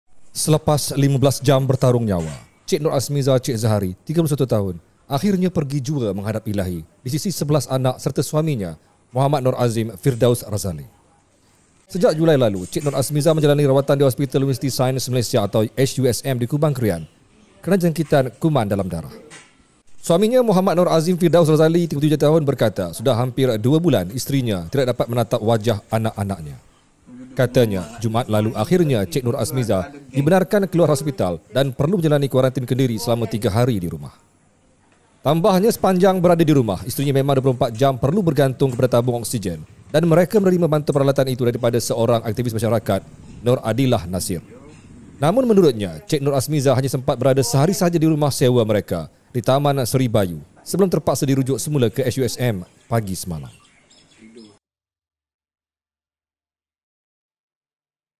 Pegang amanat arwah isteri jaga 11 cahaya mata
Selepas 15 jam bertarung nyawa, Che Nur Asmiza Che Zahari, 31, akhirnya pergi jua menghadap Ilahi di sisi 11 anak serta suaminya, Mohd Nor Azim Firdaus Razali.

Sejak Julai lalu, Che Nur Asmiza menjalani rawatan di Hospital Universiti Sains Malaysia (HUSM) Kubang Kerian kerana jangkitan kuman dalam darah.